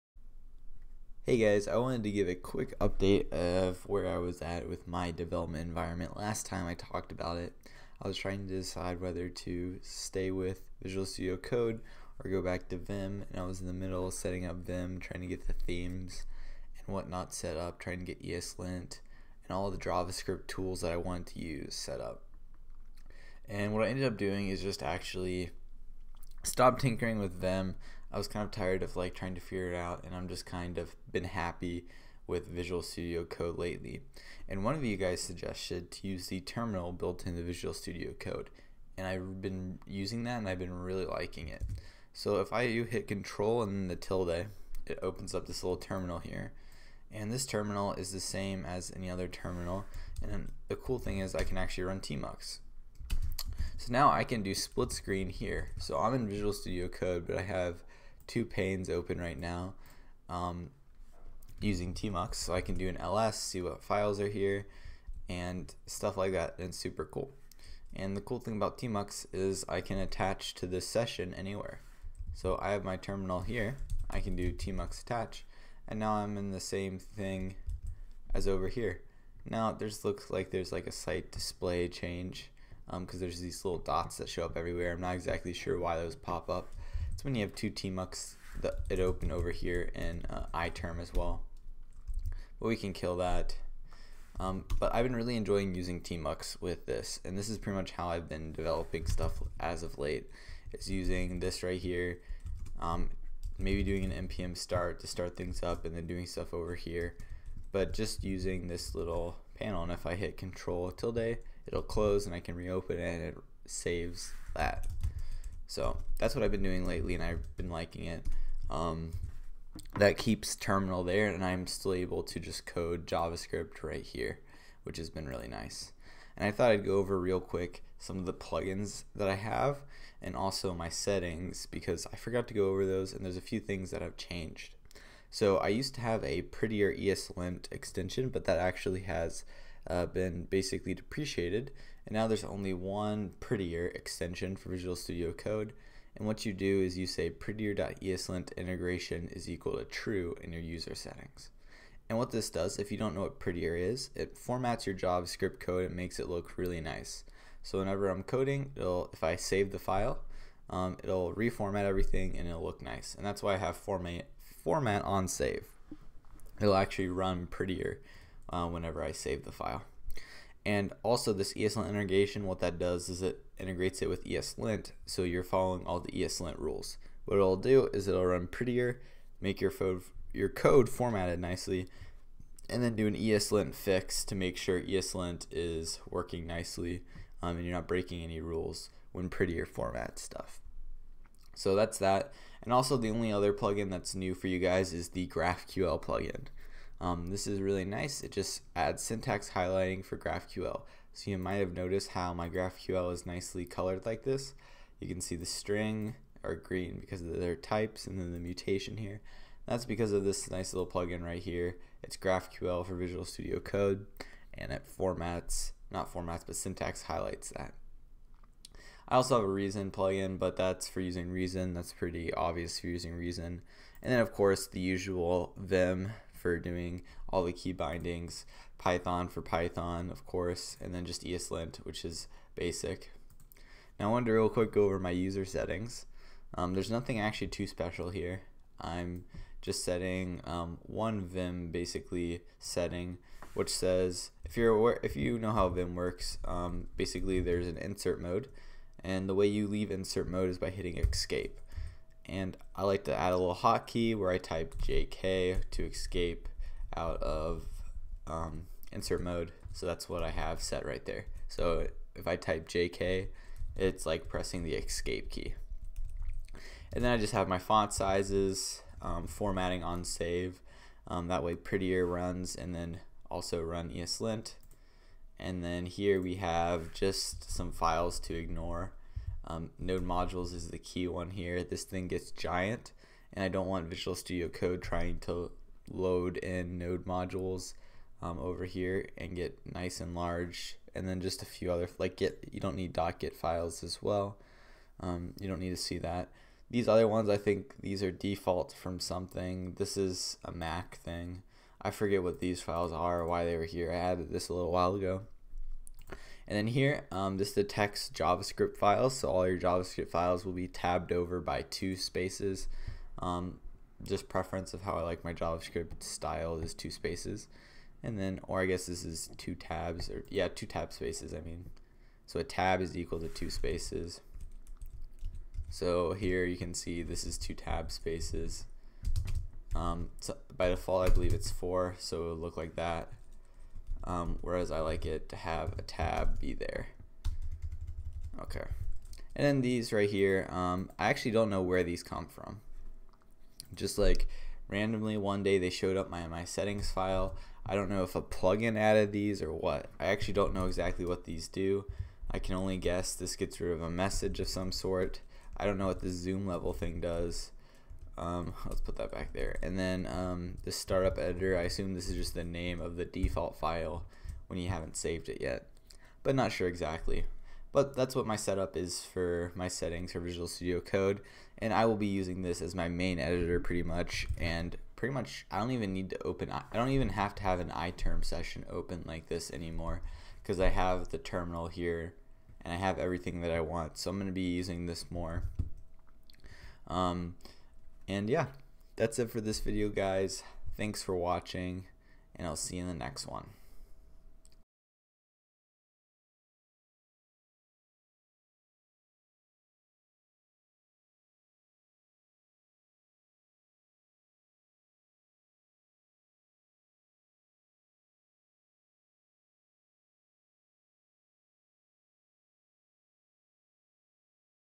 My Updated Development Setup
I discuss my updated development setup. I ended up ditching vim and just using Visual Studio Code. It was taking too long to configure and I really like using the integrated terminal in Visual Studio Code.